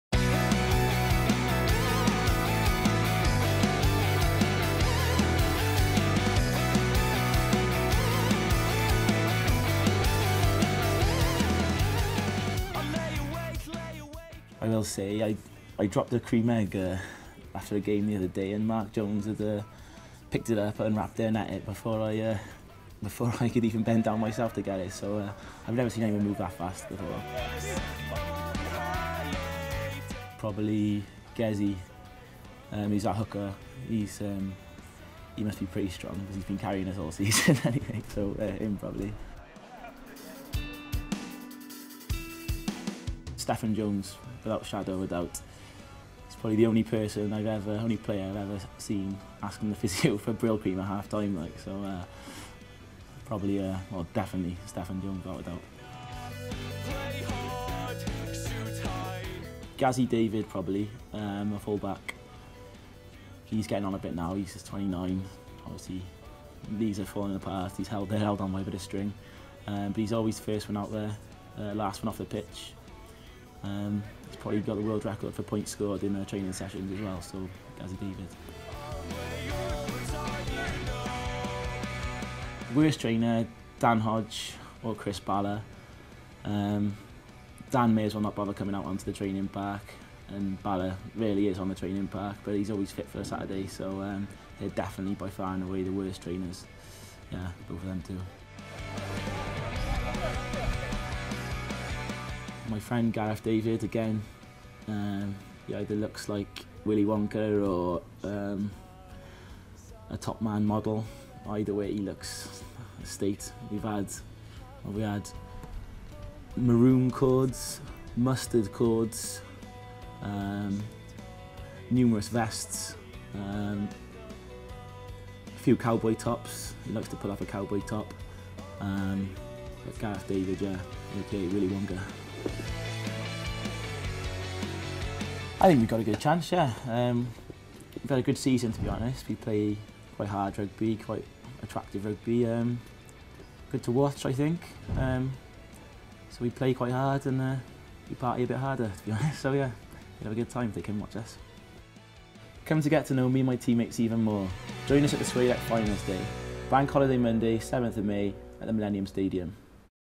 SWALEC Cup finals day: Cross Keys
Cross Keys' Leon Andrews cuts loose on his teammates as they prepare to face Pontypridd in the final of the SWALEC Cup at the Millennium Stadium on Monday.